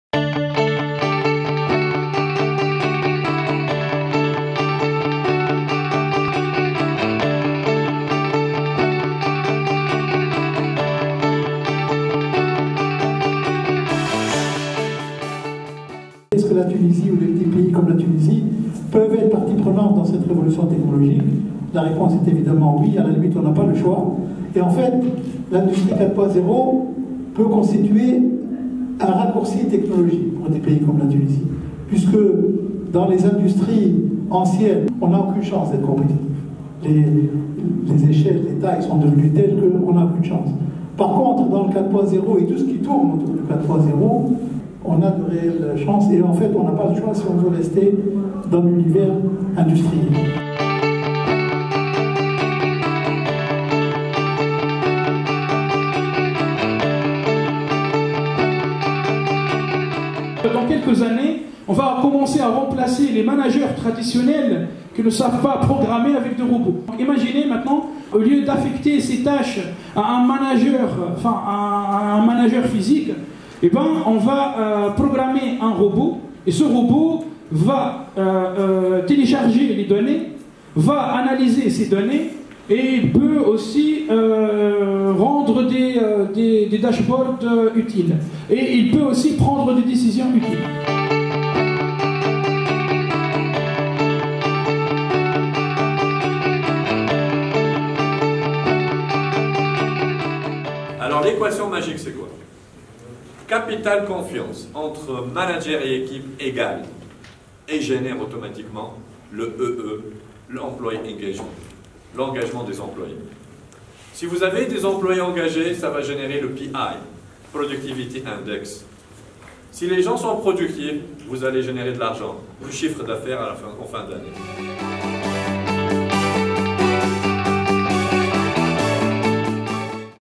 Séminaires S191125 - L1, L2, Master | M. Afif Chalbi & M. Ahmed Rebai & M. Walid Aloulou
rois conférences étaient inscrites aux programmes des séminaires d’ESB.

📍Mardi 26 novembre, M. Afif Chelbi, Président du Conseil d'Analyses Économiques, nous a invités à une réflexion autour de l'industrie tunisienne, ses difficultés et ses perspectives d'avenir à l'horizon de 2025.
📍Le mercredi 27 novembre, Ahmed Rebai, enseignant à Esprit-prépa, est revenu sur les apports de l'intelligence artificielle dans tous les domaines, ses progrès effrénés, et son impact sur l’entreprise.
📍Le jeudi 28 novembre, Walid Aloulou, general manager AIC, s'est intéressé aux styles de managers tout en rappelant les qualités requises pour le bon management d'une équipe performante.